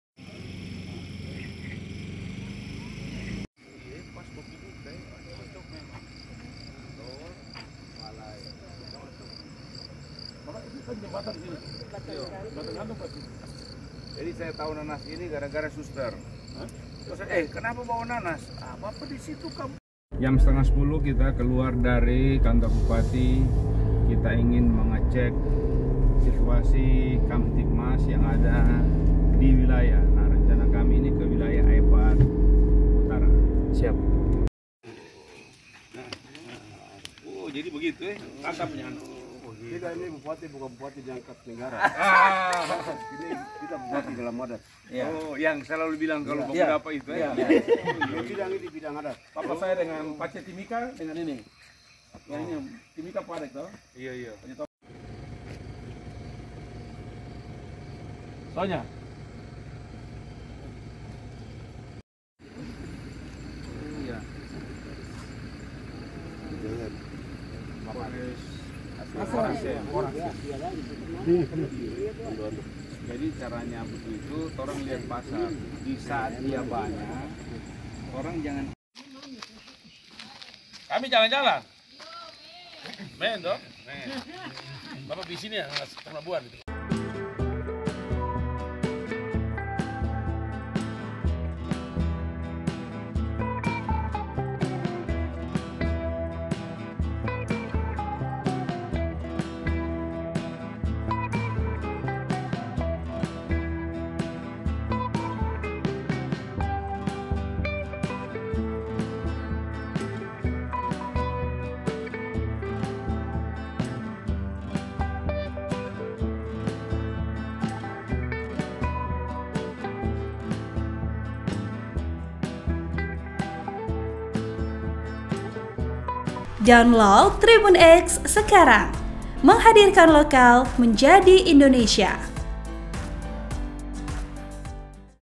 Pj Bupati dan Pj Sekda Maybrat Inspeksi Kamtibmas Sampai Larut Malam ke Kampung kampung
TRIBUNSORONG.COM, KUMURKEK - Penjabat (Pj) Bupati Maybrat Bernhard bersama Pj Sekda Ferdinandus Taa inspeksi keamanan malam hari ke beberapa kampung, termasuk Kampung Konja, Distrik Aifat Timur dan Kampung Fonatu, Distrik Aifat Utara, Kamis (8/2/2024).

Kegiatan ini bertujuan mengecek kondisi keamanan di berbagai lokasi, khususnya menjelang Pemilu 2024.

Pj Bupati Maybrat menekankan pentingnya pengecekan kamtibmas pada malam hari karena bisa mendapatkan gambaran yang akurat tentang keamanan dan ketertiban suatu daerah.

Penulis: Jariyanto | VP: Fidelis Christian Siallagan